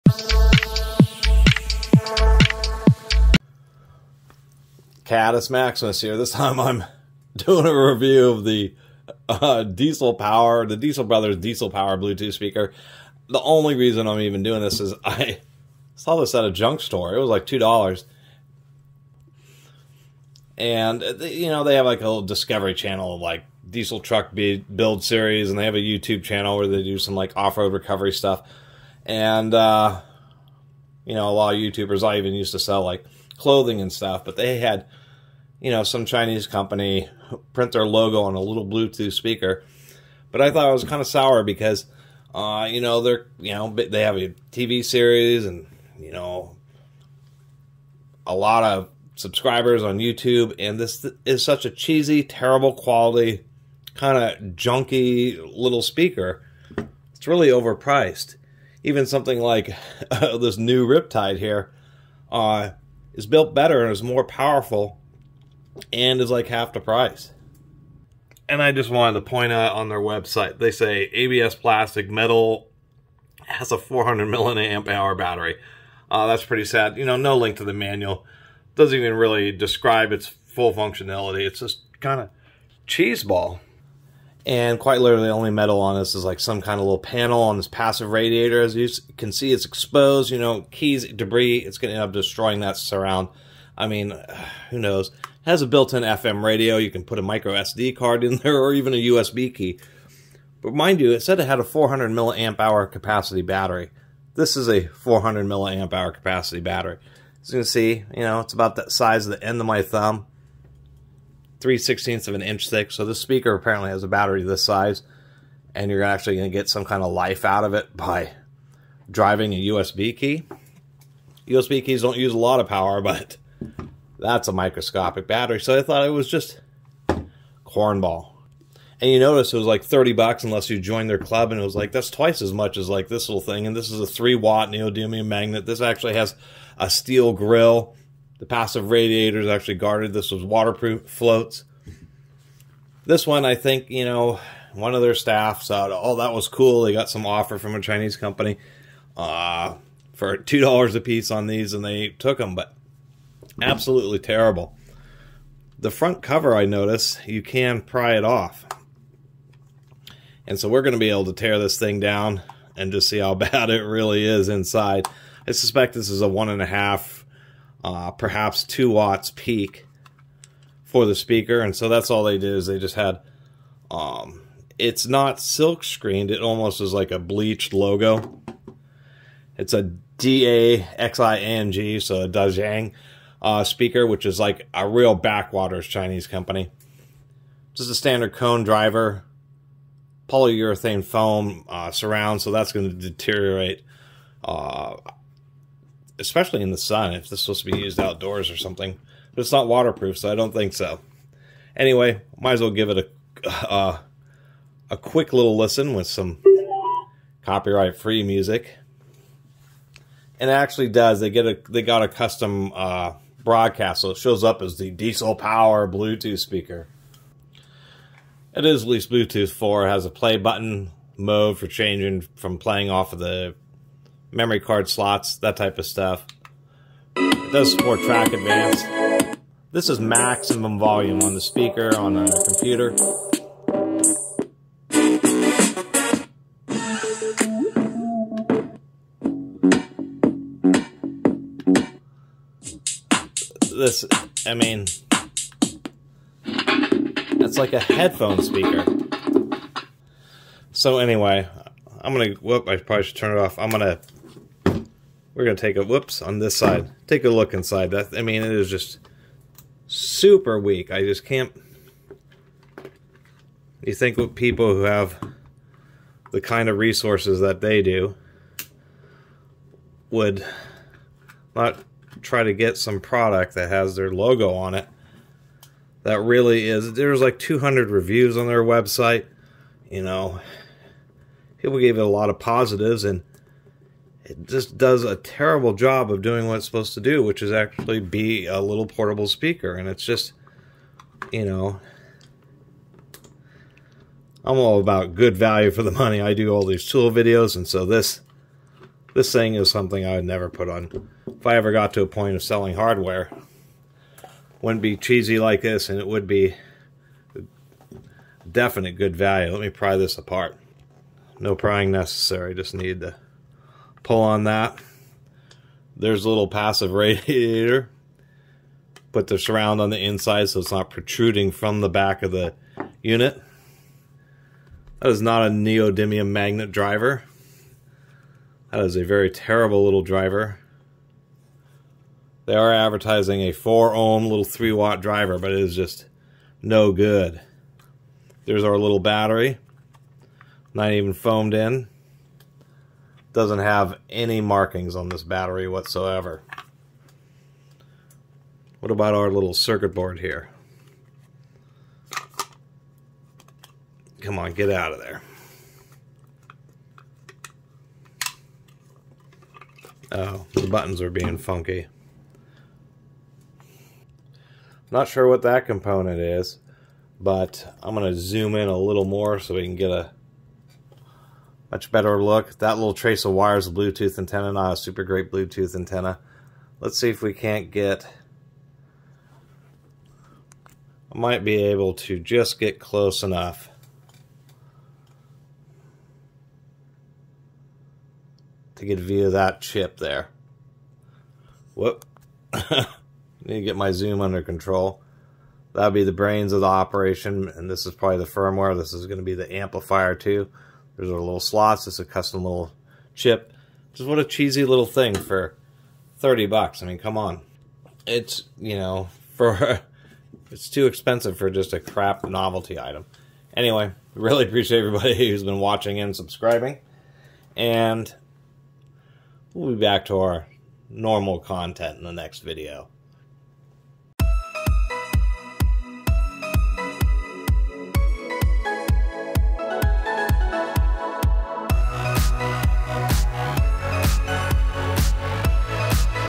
Diesel Brothers Diesel Power Blue Tooth Speaker Review/Teardown... (Disappointing!)
I randomly ran into this terrible little bluetooth speaker they are selling (or used to sell) for a ton of money and it is of the lowest performance and quality imaginable.